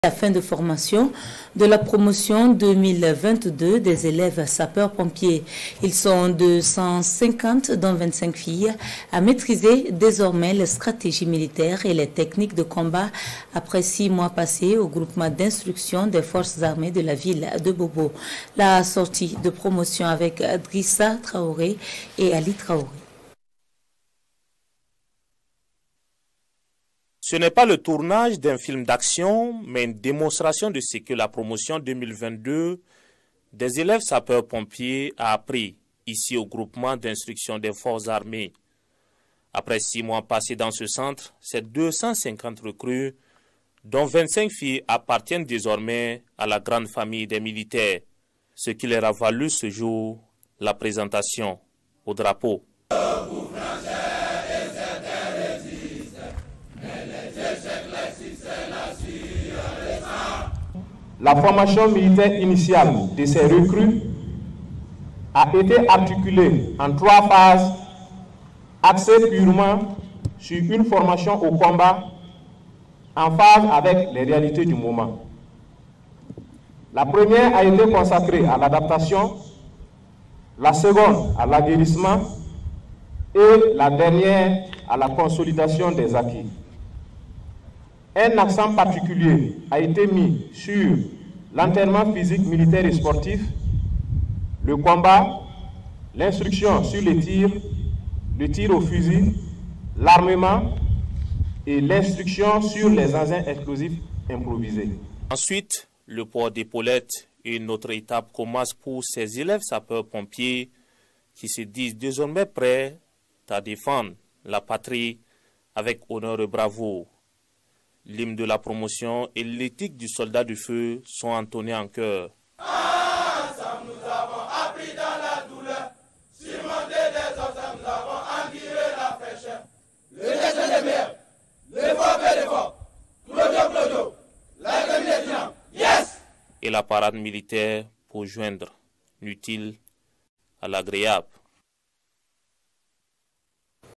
Fin de formation militaire spéciale pour les élèves Sapeurs-pompiers au GIFA
Fin de formation militaire spéciale pour les élèves Sapeurs-pompiers au Groupement d'Instruction des Forces Armées (GIFA)